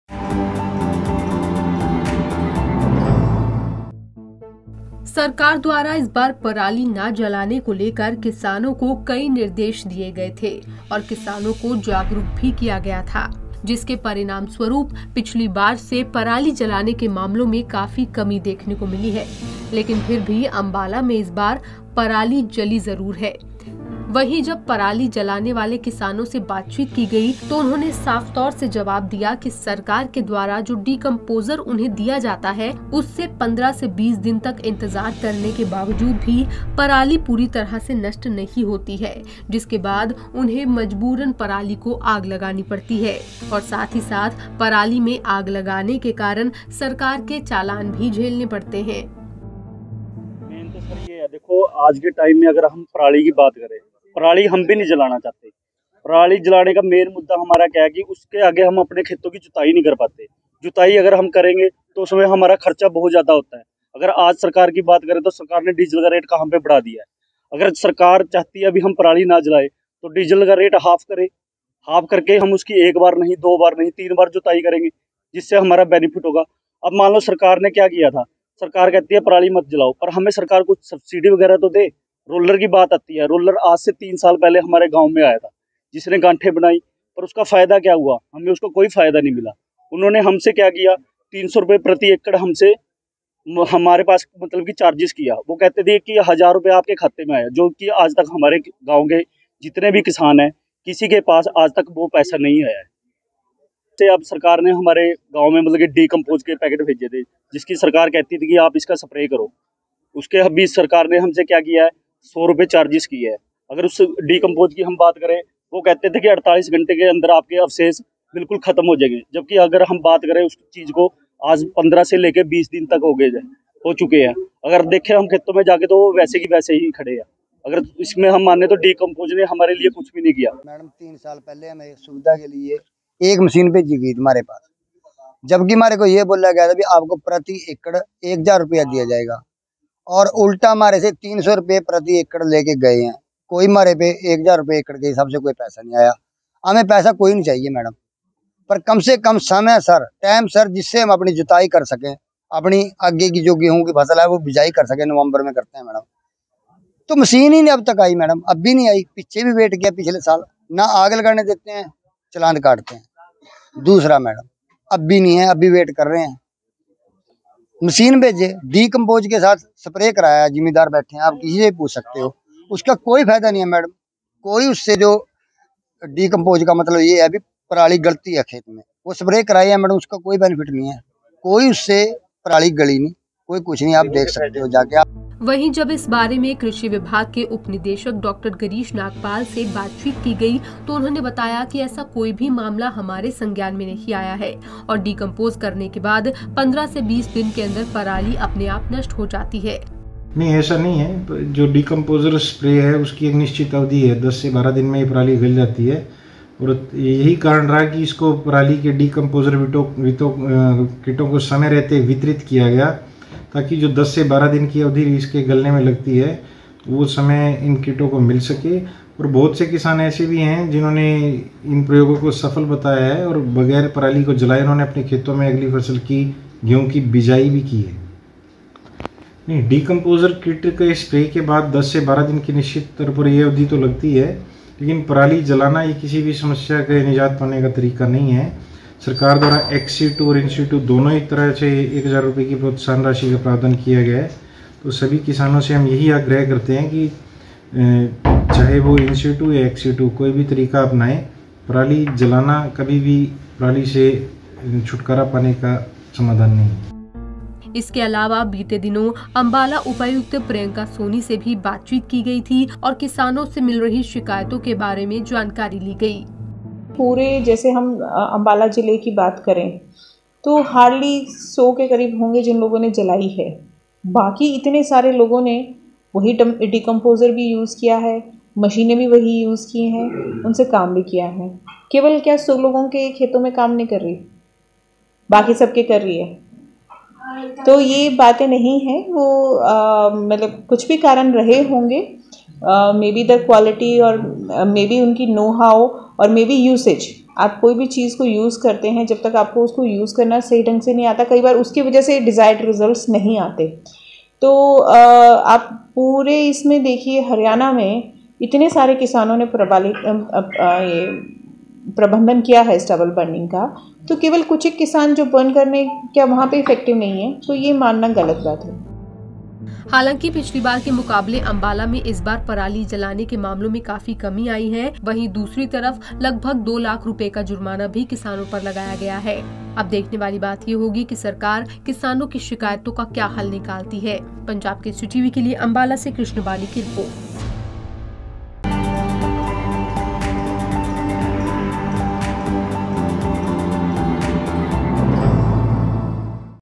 किसानों ने सरकार के डीकंपोजर को बताया खराब!, बोले- हम पराली जलाने को मजबूर
किसानों का साफ तौर पर कहना है कि सरकार के द्वारा जो डीकंपोजर उन्हें दिया जाता है उससे पराली नष्ट नहीं होती है. जिसके कारण उन्हें पराली जलानी पड़ती है.